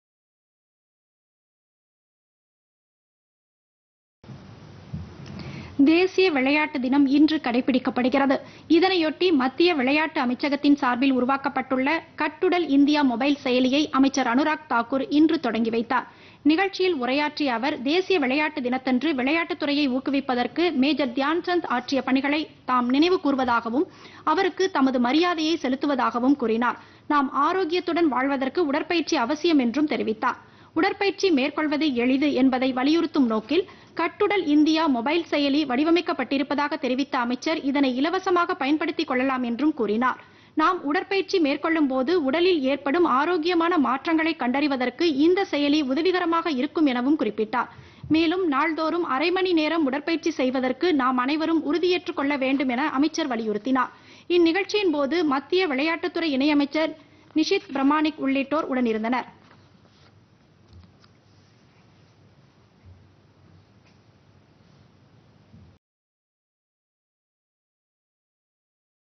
கட்டுடல் இந்தியா மொபைல் செயலி தொடக்கம்PodhigaiTamilNews
தேசிய விளையாட்டு தினம் - கட்டுடல் இந்தியா மொபைல் செயலியை மத்திய விளையாட்டுத் துறை அமைச்சர் அனுராக் சிங் தாகூர் தொடங்கி வைத்தார்.